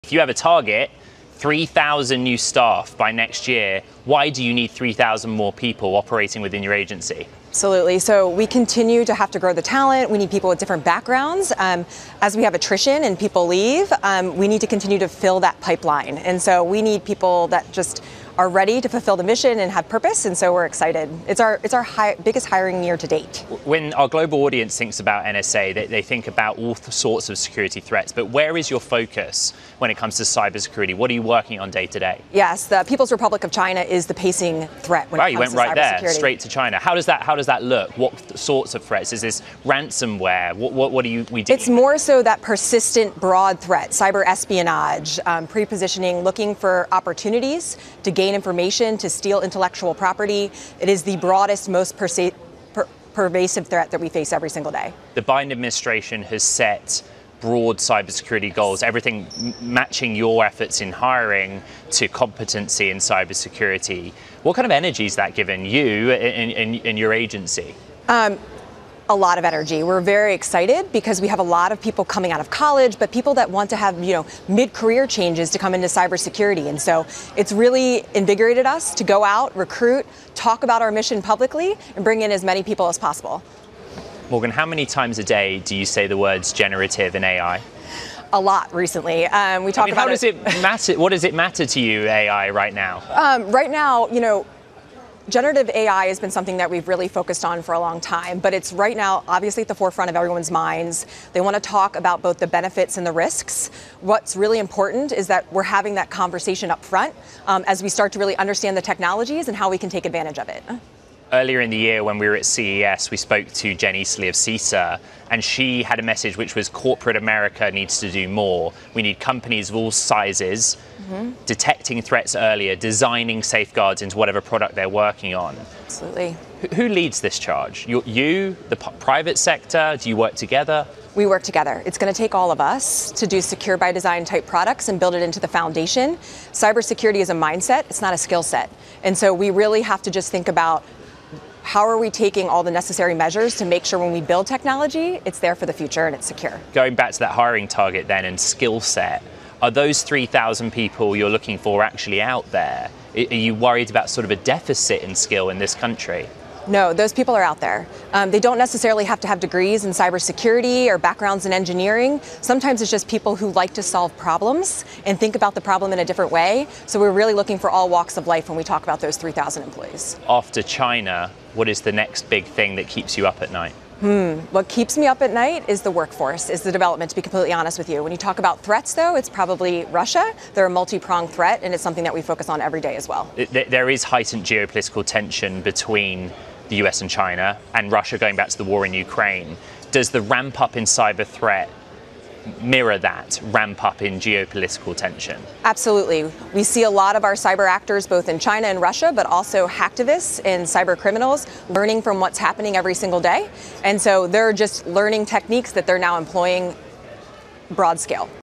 NSA Wants to Hire 3,000 Cyber Warriors
NSA Cybersecurity Collaboration Center Chief Morgan Adamski joins Ed Ludlow live from the RSA conference to discuss the agency's historic initiative to hire 3,000 new employees by next year.